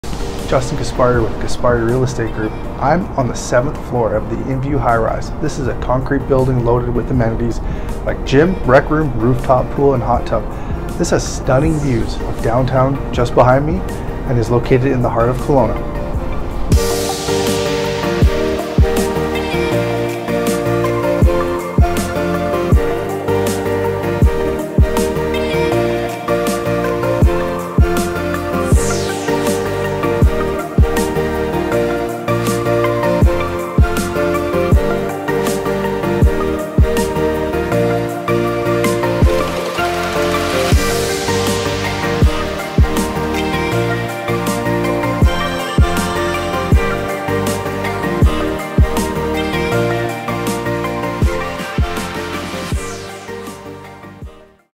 708-2040 Springfield Road - Kelowna Condo For Sale
SOLD! 708-2040 Springfield Road
2 Bedrooms, 2 Bathrooms | 1010 Sq Ft

Discover urban convenience in this exquisite 2-bedroom, 2 full-bath condo nestled on the 7th floor of InVue. Located on the quiet side of the building on the 7th floor, enjoy panoramic mountain and city views from TWO balconies, including a private deck off the primary bedroom. Open-concept kitchen with island, pantry, and stainless steel appliances. Geothermal heating and cooling provides energy-efficient and sustainable living. Luxurious, resort-style amenities include a rooftop pool, hot tub, BBQ area, sauna, pool table room, dog wash station, guest suite, and a well-equipped gym. Monthly Strata fees cover a wide range of services, including heat, air conditioning, water, recreational facilities, and insurance, ensuring a hassle-free living experience. Convenient, walkable location in Central Kelowna. This is urban living at its finest!

Listed by Gaspari Real Estate | Coldwell Banker Horizon Realty.